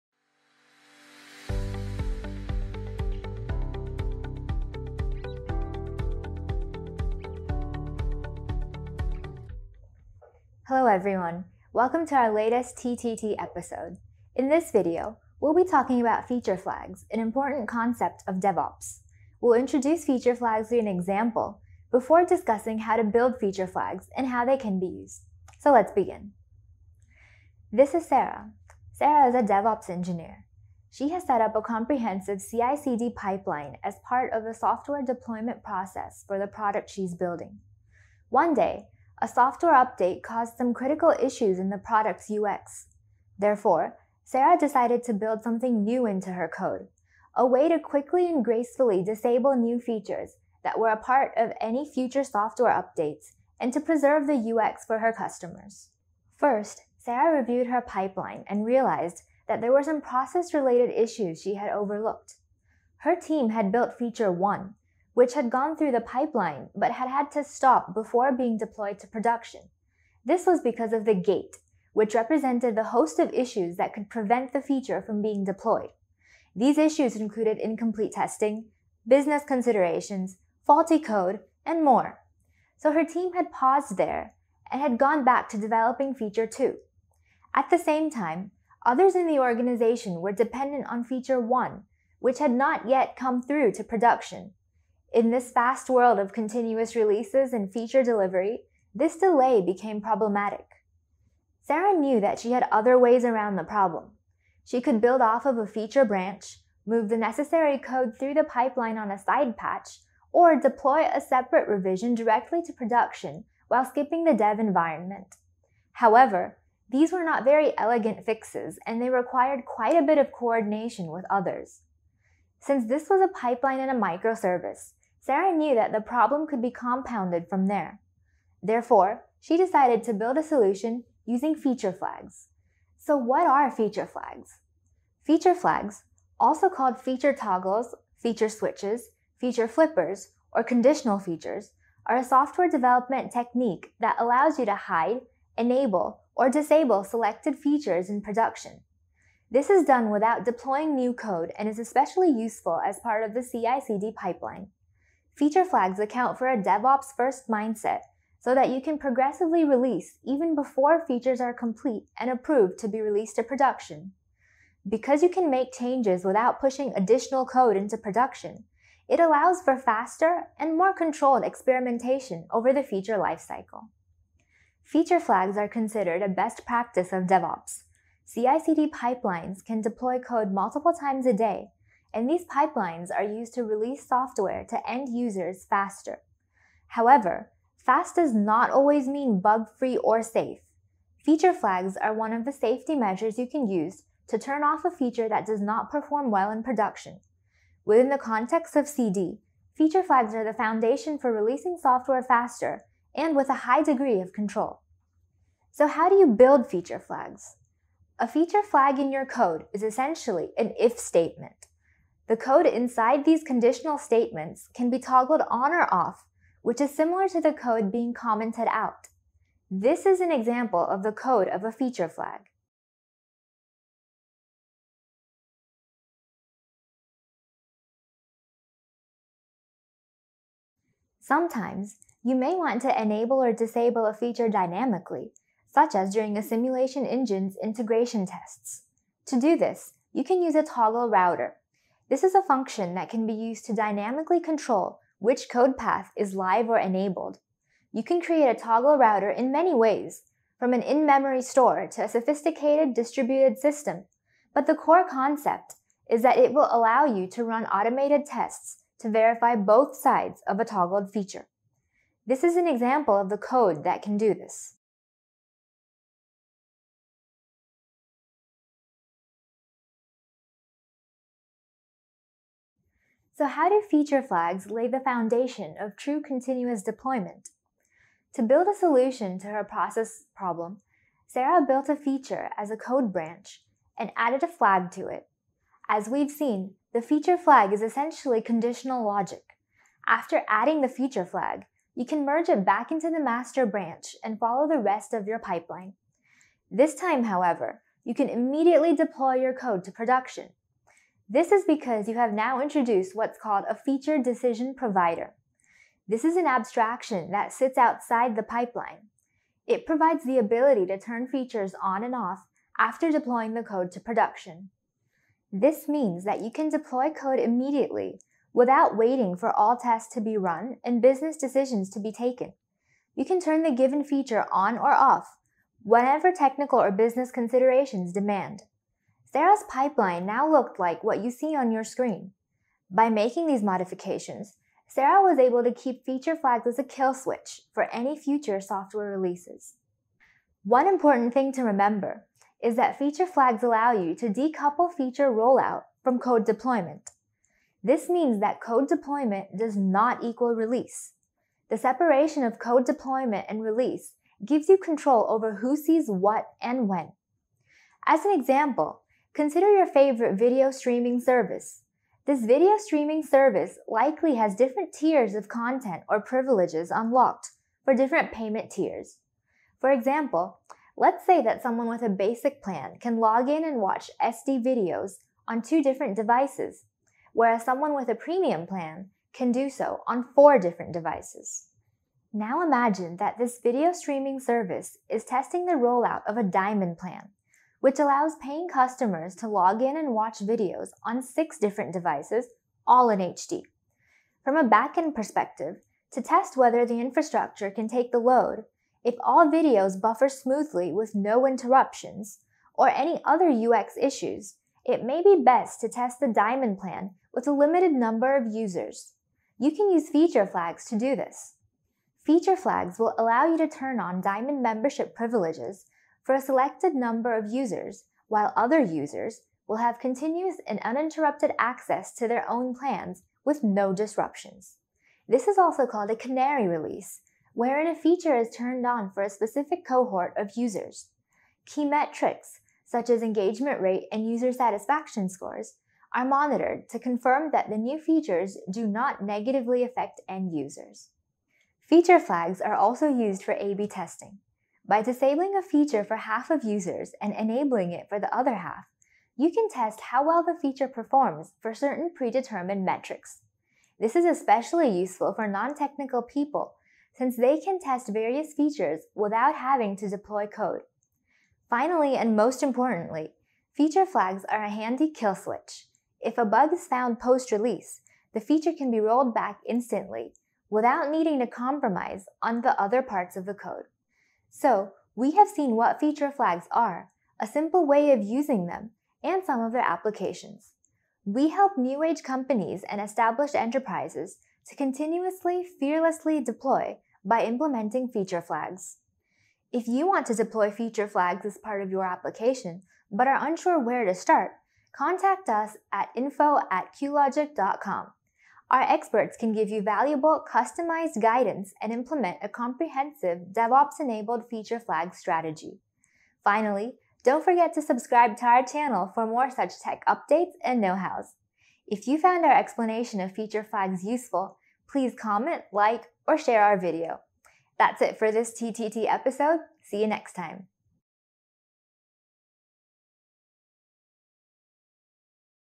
Feature Flags | Feature Toggles | TTT | Cuelogic
In this episode of TTT, we talk about Feature flags (or feature toggles) which is one of the hottest topics in Software Development. It lets you incrementally roll out functionality in a more controlled way.

It is a switch that you can use to turn a feature on or off within an application after the application has been deployed to production. Hence, no more “hope, we don’t run into a problem” daunting deployment situations.

In this guide on Feature Flags, we cover what are Feature Flags as well as take you through an example where a developer uses feature flags to solve a process and feature release problem. We also provide some examples of code that can help you build feature flags.

The video will help to uncover some commonly asked questions like; what are feature flags, how are they used and how have they come to form the foundation of Continuous Delivery in DevOps.

Looking to release faster, but with better control? Our DevOps services can help.

0:00 Intro
0:26 Example
2:12 Feature Flags Concept
3:26 Building Feature Flags
4:34 Feature Flag Solution
7:54 Feature Flags at Cuelogic
8:26 Outro